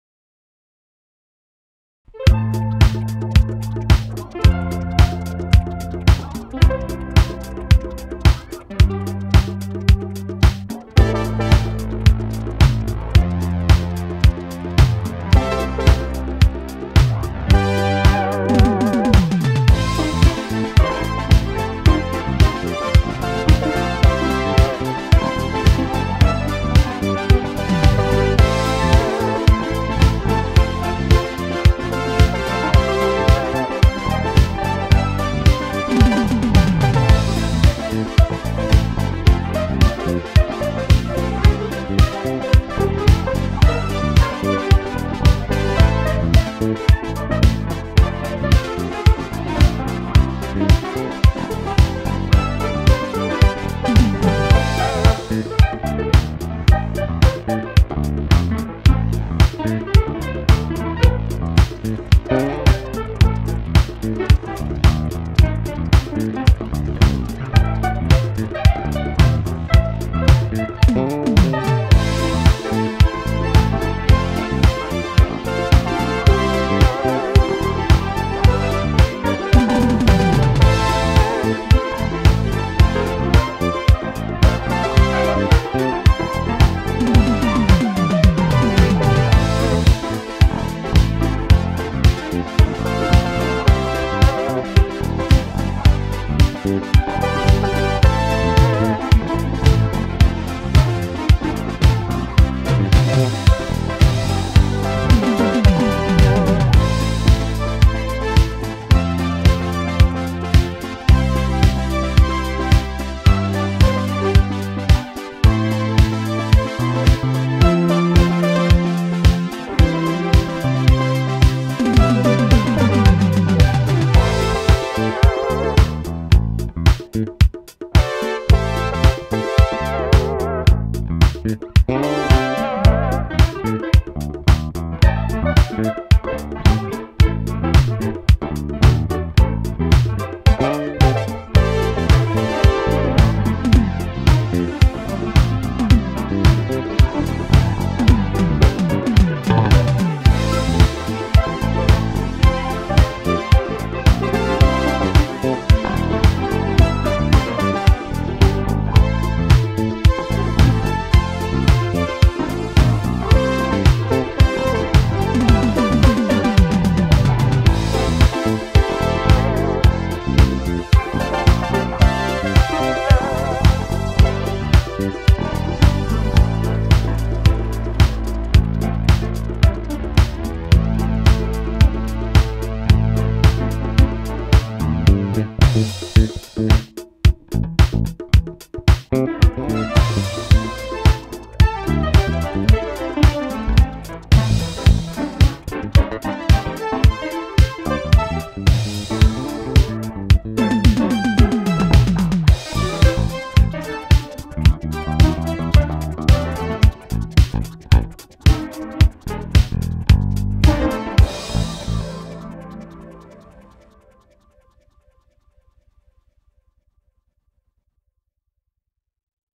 SIEL DK 80 Italo synth
A demo of the SIEL DK 80 , a DCO-based Italian synth from 1985.  The LinnDrum provides the drum beat, a Yamaha RBX170 provides the bass line, and the DK80 does all the other sounds.

The style is Italo Disco / Synth Pop of the 1980s.